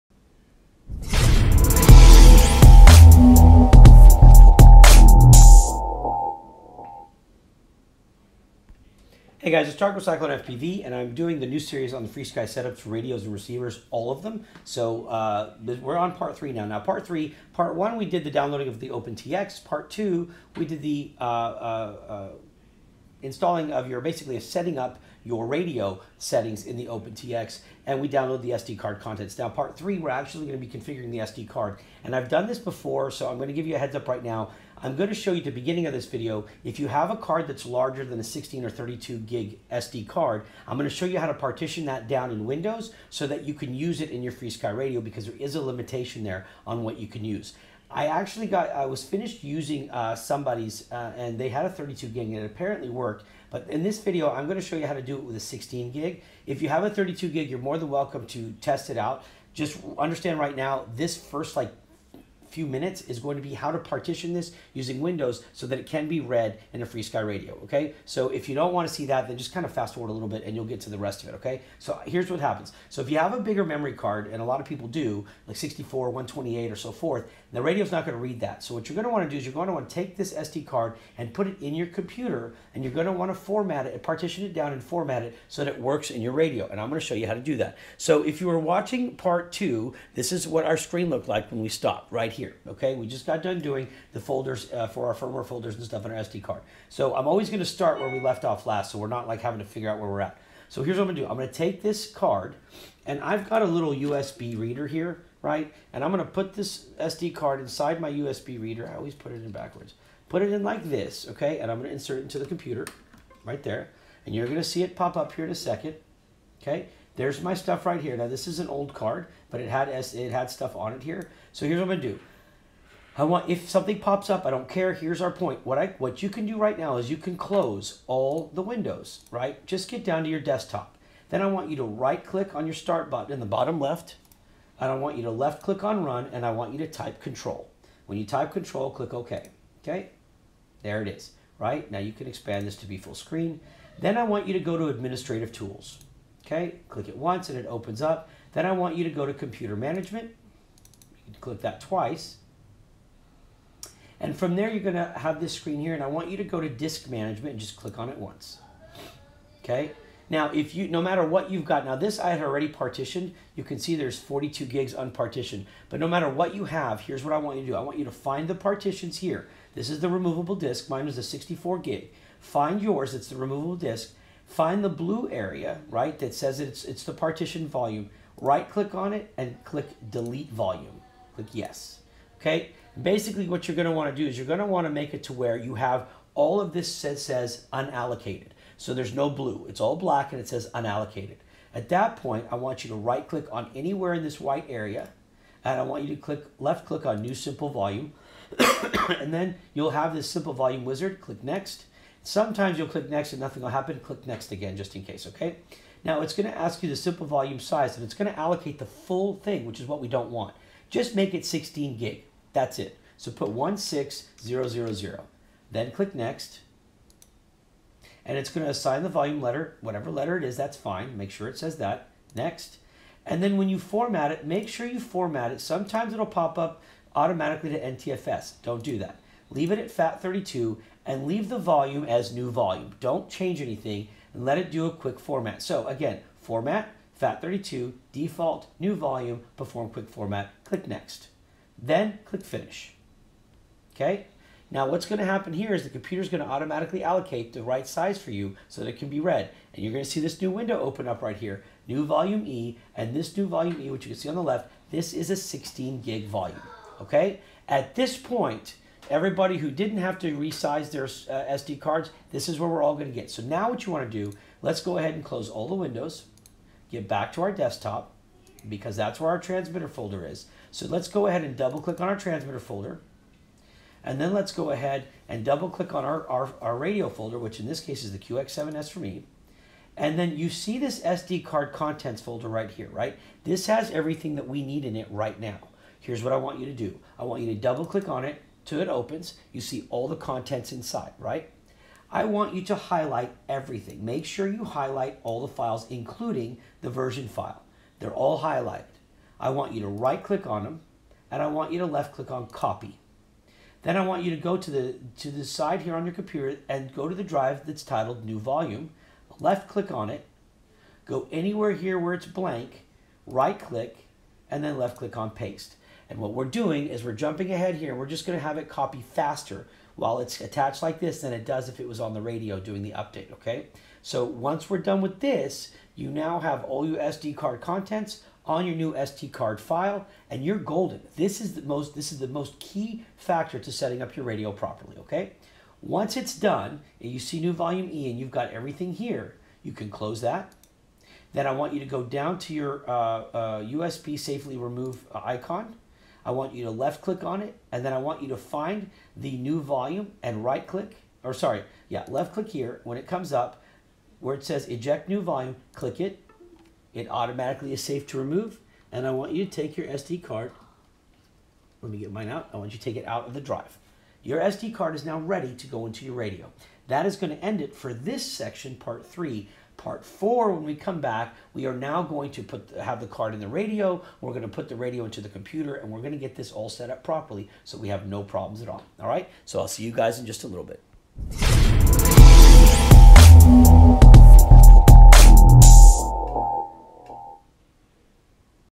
Complete FrSky Setup Guide for All Radios - Part 3 SDCard Setup
I am updating the entire series of FrSky radio and receiver tutorials that I have done over the years. The new updates will be easier to understand and will allow for the new ACCESS protocol information as well as the new receivers like the Archer series that have been released. I am also going to divide the videos into smaller sections and hopefully move at a slower pace so that people quit bitching at me for moving too fast. I get it. I am working on it. I hope this helps. Please let me know if you need other help and I will make a new video for you as well.

Sincerely,

Tarek Maalouf
“RitalinFPV”
Work Mule
CycloneFPV.com